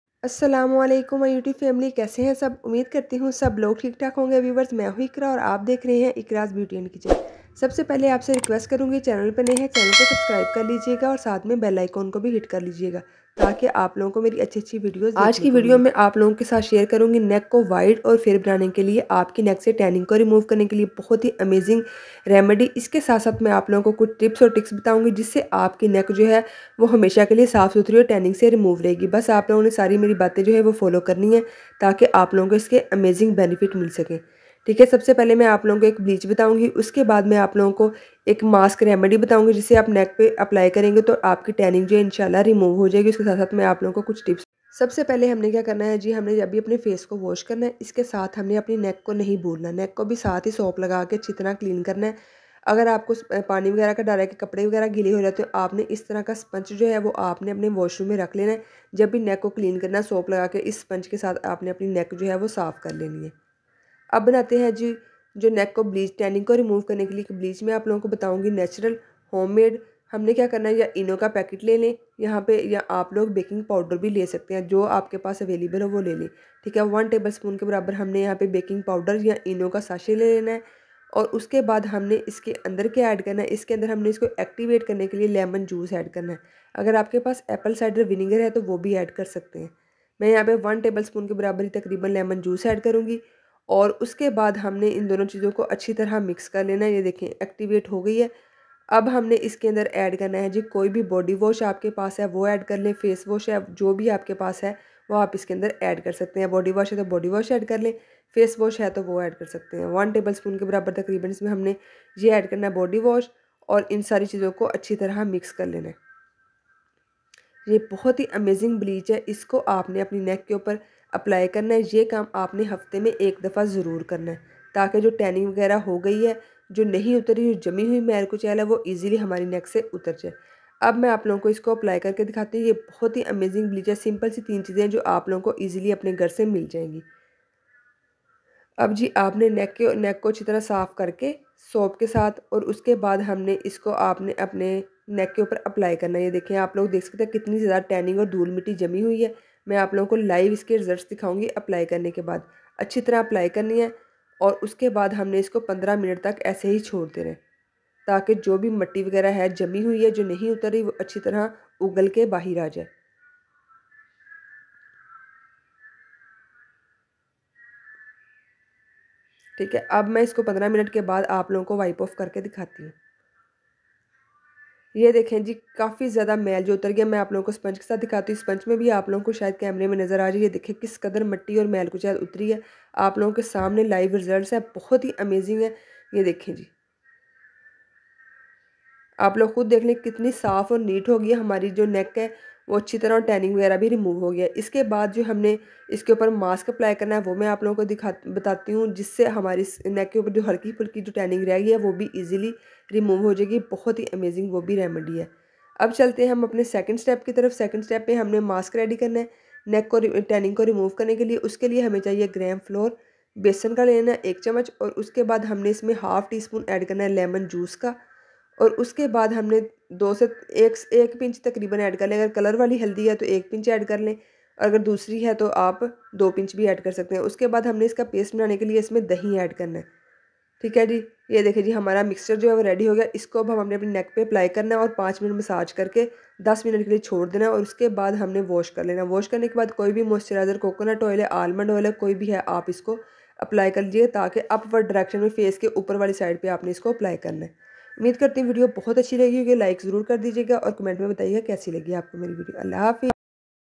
Get Rid of Dark Neck in 20 Minutes -/NECK WHITENING AT HOME /Iqras Beauty and Kitchen
Get Rid of Dark Neck in 20 Minutes -/NECK WHITENING AT HOME /Iqras Beauty and Kitchenn Today's Video, I will be sharing a wonderful remedy which will cure your dark neck, dark elbows, dark angle and dark knee problems in just 7 DAYS. It is a wonderful remedy for those who are  distressed with their skin issues and looking for a genuine and easy effective remedy to get rid of dark area around neck, elbows.

Use the Remedy every day for at least 7 days as shown in the video to get the desired results.

If you have very dark neck issue, you can continue using it for 2-3 weeks to get the desired results